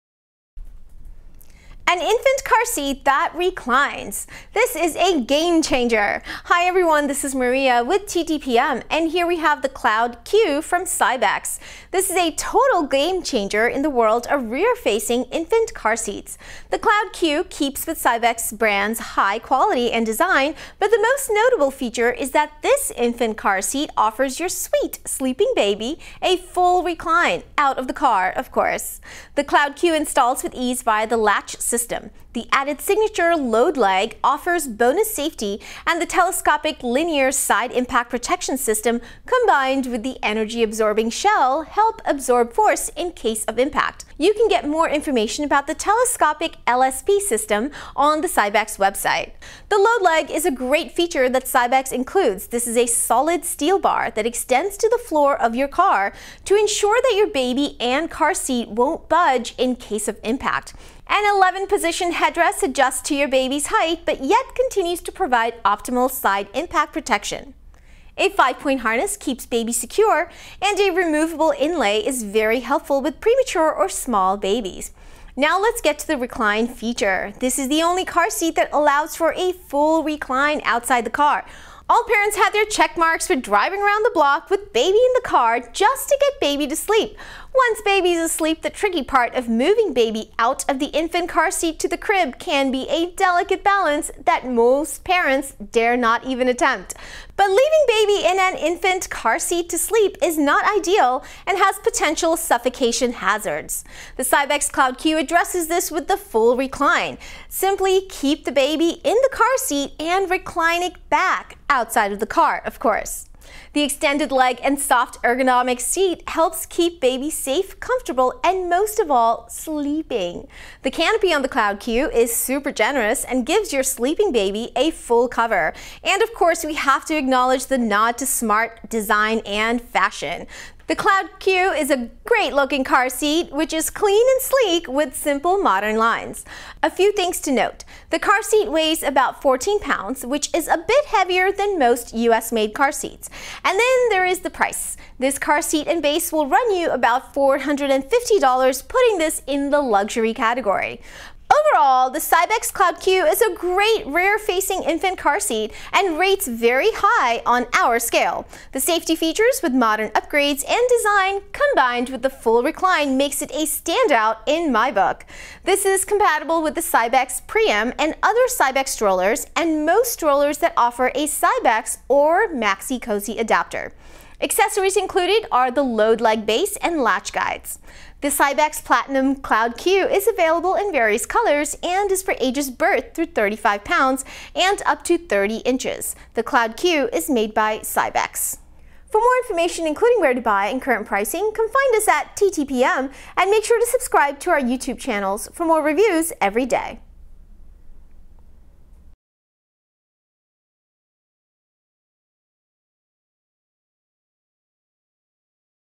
Cloud Q Infant Car Seat + Base from Cybex Platinum
Cloud Q Infant Car Seat and Base from Cybex Platinum! Safe ergonomic design! Foldaways XXL canopy with UPV50+ sun protection! 11-point adjustable headrest! Here's an innovative, extremely safe car seat that adjusts to your baby's height as they grow. For full review and shopping info

Product Info:
A comfortable, ergonomic design, the Cloud Q Infant Car Seat and Base is the result of ongoing research at Cybex Platinum. Building on previous infant car seats, the car seat offers a full-recline position outside of the car, a foldaway XXL canopy with UPV50+ sun protection, and 11-position height-adjustable headrest, Telescopic Linear Side-Impact Protection (L.S.P. system), and the removable newborn inlay molded for premature and very small babies. This protects your baby from all areas at all stages of development. There's even a solid steel bar that goes under the actual seat of the car itself for added safety during installation.

The Cloud Q is also travel ready and can be added to Cybex and other brand strollers. By researching with doctors, midwives, and numerous test results, this car seat fully reclines and molds to the shape of your child's body, head, and neck. The L.S.P. system, flexible material, structure, and energy absorbing shell of the car seat make sure the forces of impact are channeled away from babies—a precautionary feature that should never have to be utilized.